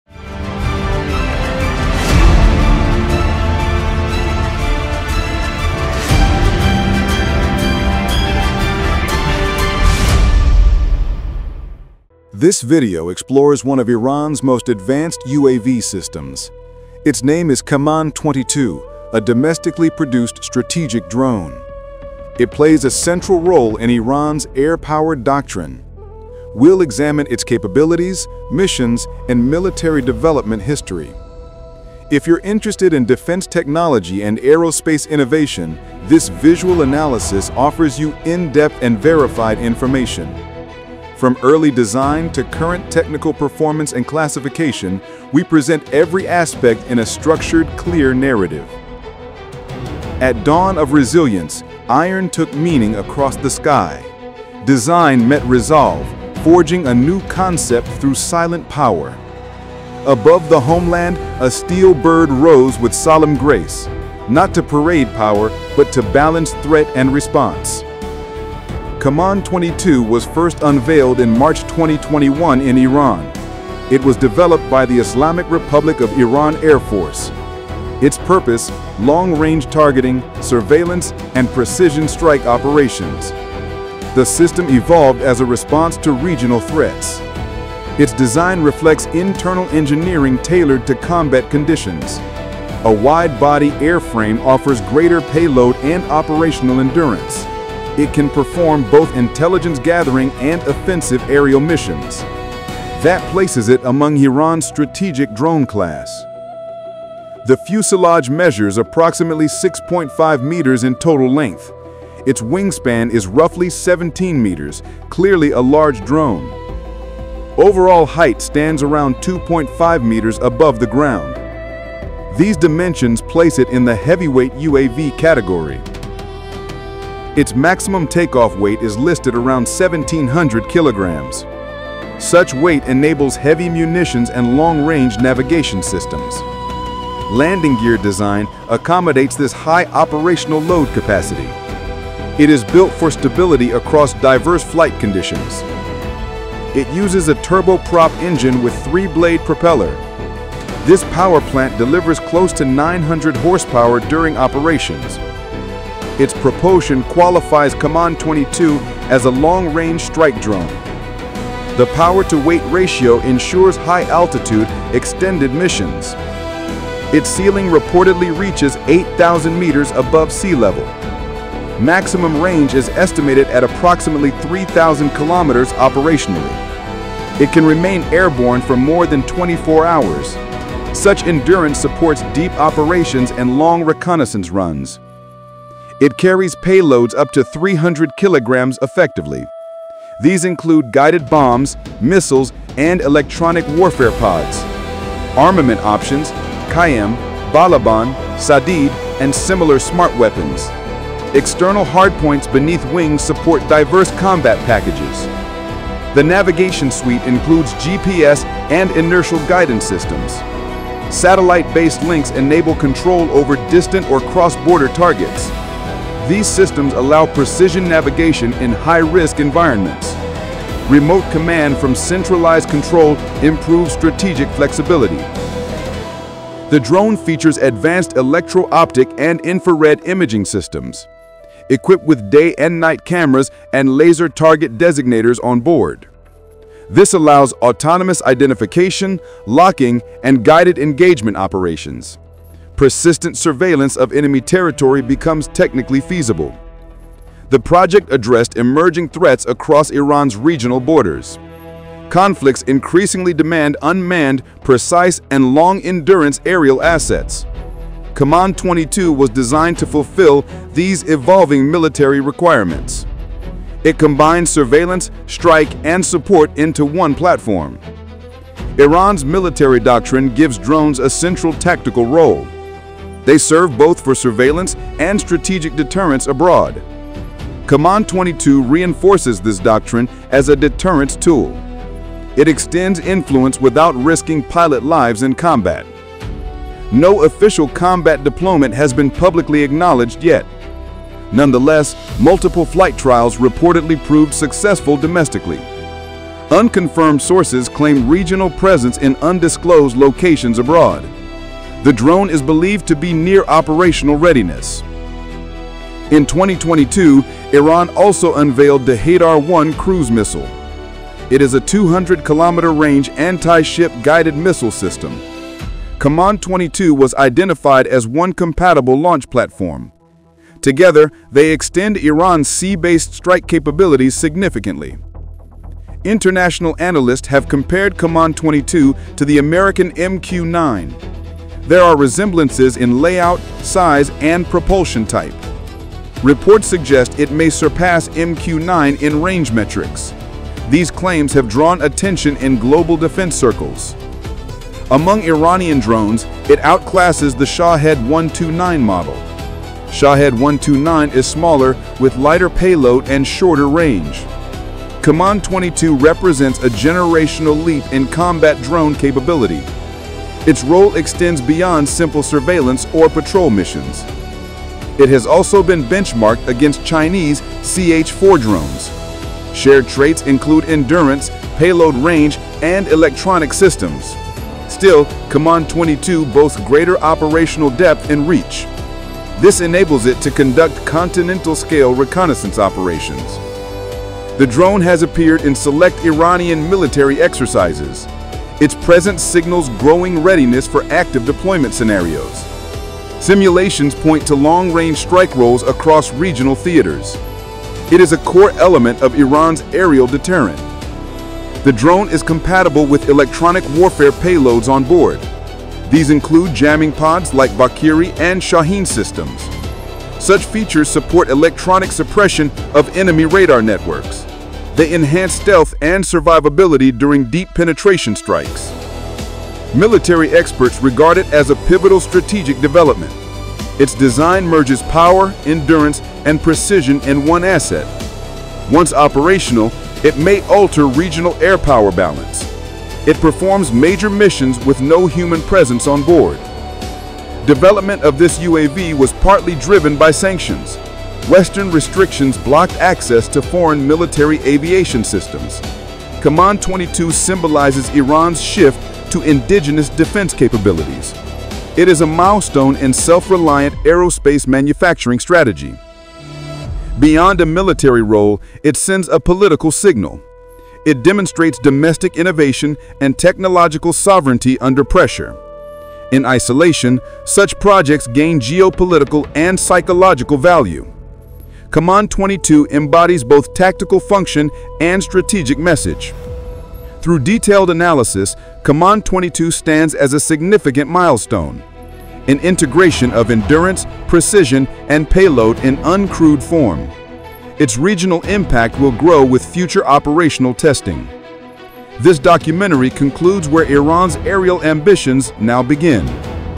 Iran Unveiling Kaman-22: The Dawn of Iran’s Next-Gen Air Force
Kaman-22 is Iran’s largest and most advanced strategic UAV, unveiled in 2021 by the Islamic Republic of Iran Air Force. Designed for long-range surveillance and precision strikes, it can fly over 3,000 kilometers and stay airborne for 24 hours while carrying up to 300 kilograms of payload. Equipped with GPS guidance, electro-optical sensors, and smart munitions, it symbolizes Iran’s push for self-reliant defense under sanctions. Often compared to the American MQ-9 Reaper, Kaman-22 represents a generational leap in Iran’s drone capability and a key pillar of its aerial deterrence doctrine.

This video is visual created using A.I 3D Tools. It is for informational and educational purposes only. All visuals is synthetic and not based on real events